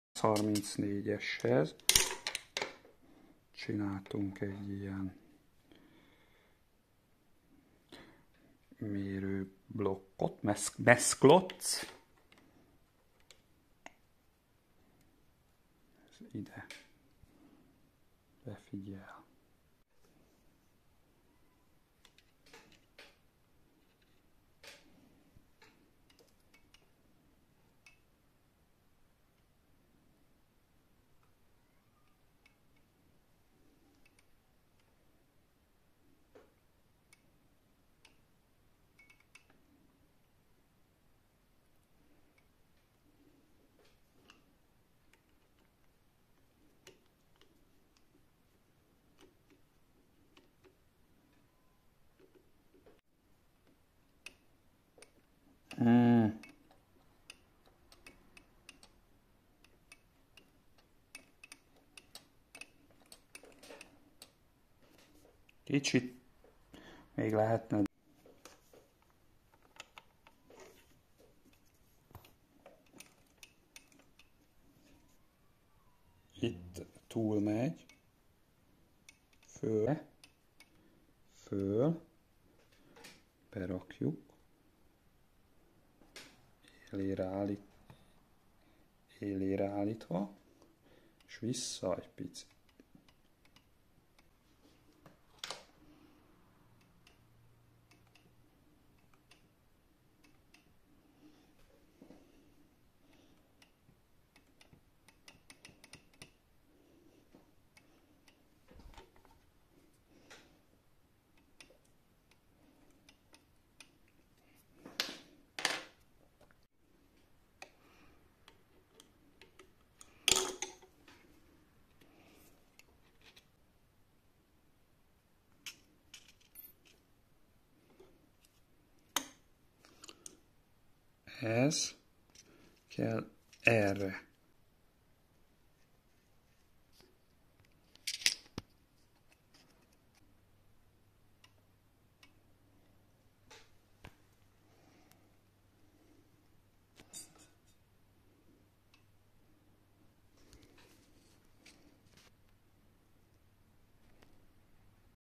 Textima Altin hook timing adjustment
Tool dimensions:
"cube" size: 64x10.22x12.02mm for 134 / 135x5 needle system

(64x10.22x8.22mm for needle system 135x1)

pin size: l=15.8 d=2mm